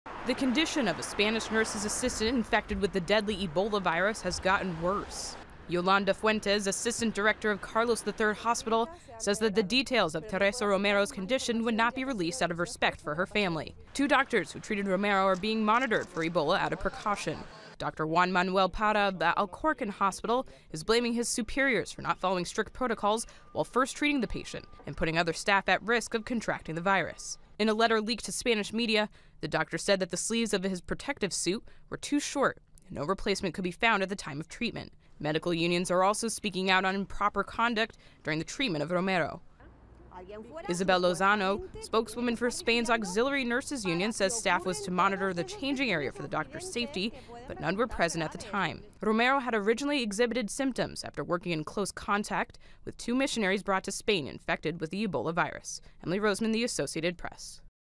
Medical Staff Speak Out Against Ebola Protocols
The staff treating a patient with the Ebola virus are speaking out against improper protocols taken by hospital superiors in preventing the spread of the disease to staff. (Oct. 9)